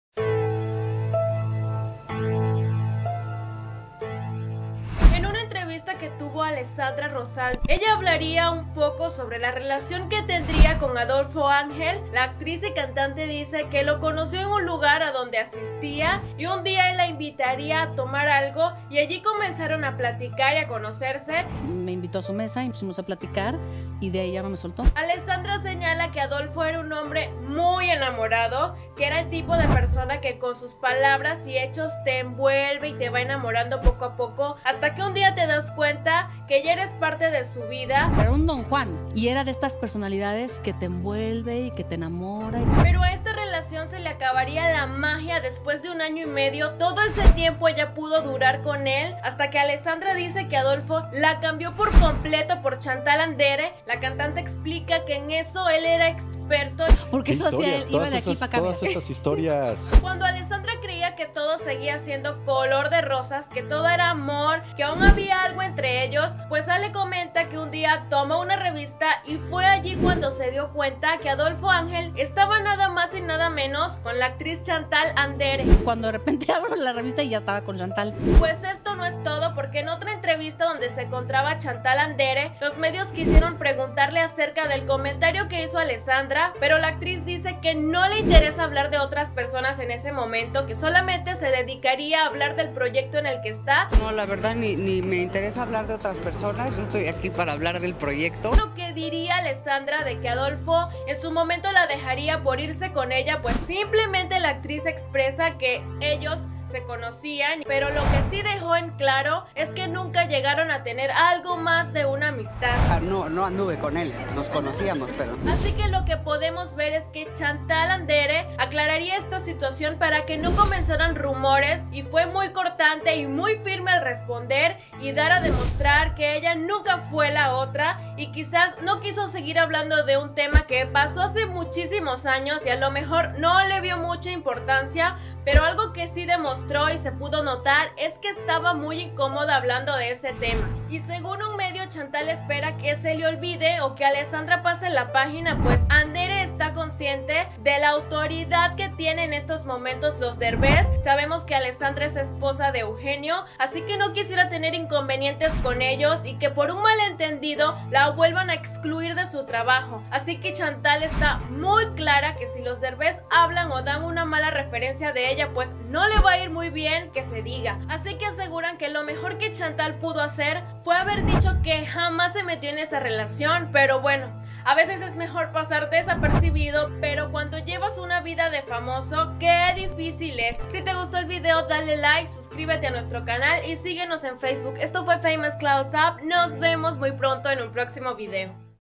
⛔🔥 Alessandra Rosaldo 𝗥𝗘𝗩𝗘𝗟𝗔 que Chantal se entrometió en su relación
Esposa de Eugenio Derbez recuerda nostálgica y sonriente su romance con Adolfo Ángel “El Temerario”